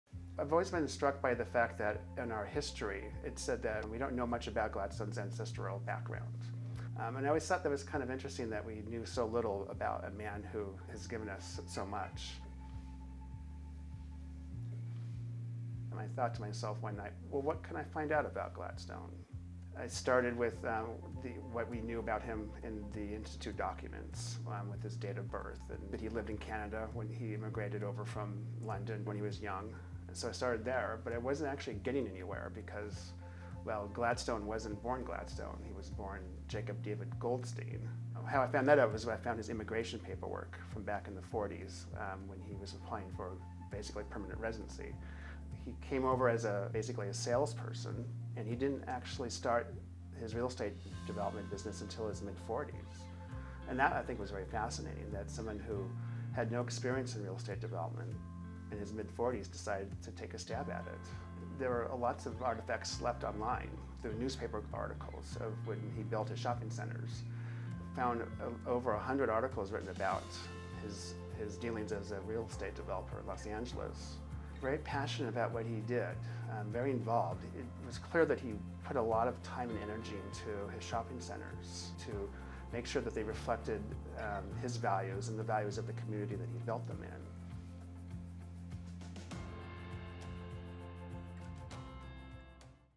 Discovering J. David Gladstone
Jack David Gladstone was the self-made American man, a Horatio Alger immigrant hero for the 20th century with a sprawling, if enigmatic life story. Orphaned as a young boy, he emerged later in life as a true pioneer in real estate development before generously bequeathing a trust that has grown dramatically over the past 40 years, supporting visionary life science research to tackle unsolved diseases and improve the global human condition.

His posthumous generosity continues to sustain the Gladstone Institutes’ lofty mission, netting astonishing successes across multiple disciplines, much of it beyond the imagination of the forward-thinking Gladstone, who reinvented his life with a different name on another continent.